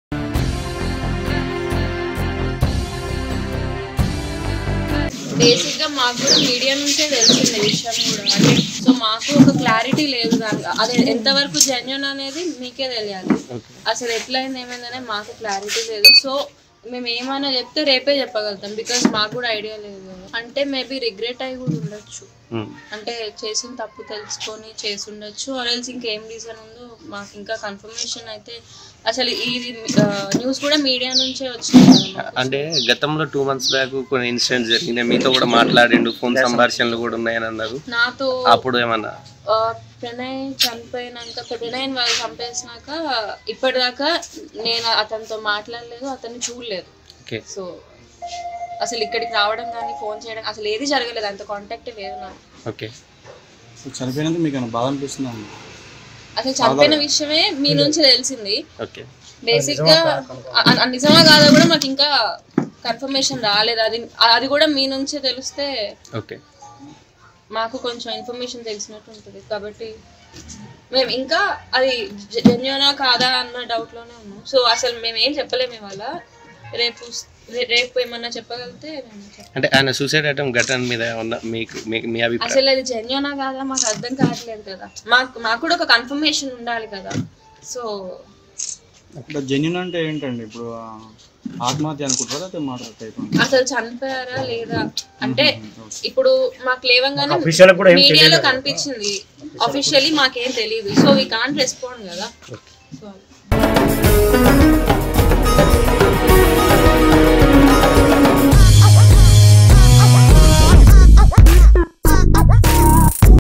మారుతీ రావు మృతిపై అమృత రియాక్షన్ చూడండి || Pranay Amrutha On Maruthi Rao Incident
Watch Pranay Amrutha On Maruthi Rao Incident By Jabardasth News.